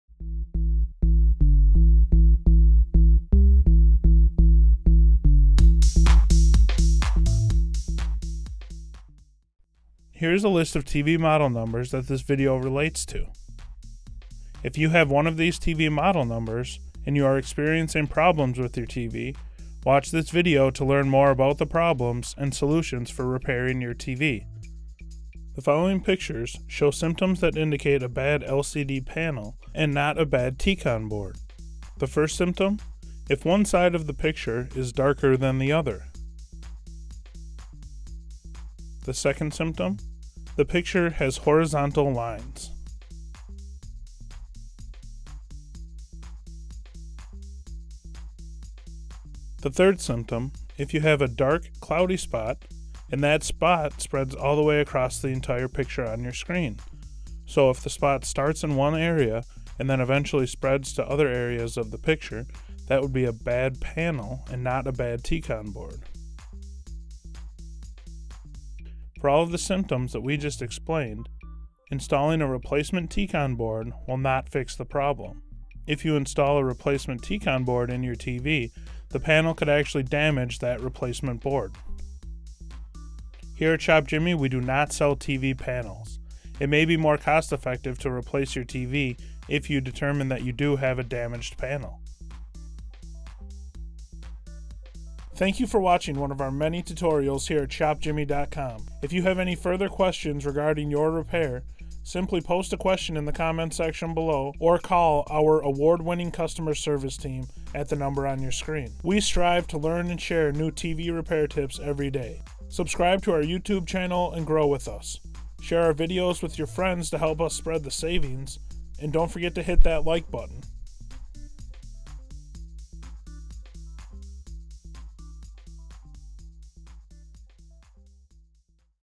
Vizio LED Screen Repair T-Con or Bad Panel Tutorial & Help - E601I-A3 E601I-A3E E701I-A3 E701I-A3E
Vizio Screen Problem T-Con Problem or Bad Panel Tutorial & Help  for the following models E601I-A3 E601I-A3E E701I-A3 E701I-A3E

Here is a list of TV model numbers that this video relates to.
If you have one of these TV model numbers, and you are experiencing problems with your TV, watch this video to learn more about the problems and solutions for repairing your TV.
The following pictures show symptoms that indicate a bad LCD and not a bad T-con board.
The first symptom, if one side of the picture is darker than the other.
The second symptom, the picture has horizontal lines.
The third symptom, if you have a dark cloudy spot and that spot spread all the way across the picture on your screen.  So, if the sport starts in one area and then eventually spreads to other areas of the picture, that would be a bad panel and not a bad T-Con board.
For all of the symptoms that we just explained, in stalling a replacement T-con board will not fix the problem.  If you install a replacement T-Con board in your TV, the panel could actually damage that replacement board.
Here at ShopJimmy, we do not sell TV panels, it may be more cost-effective to replace your TV if you do determine that you do have a damaged panel.
Thank you for watching one of our many tutorials here at Shopjimmy.com.  We strive to learn and share new TV repair tips everyday, subscribe to our youtube channel and grow with us.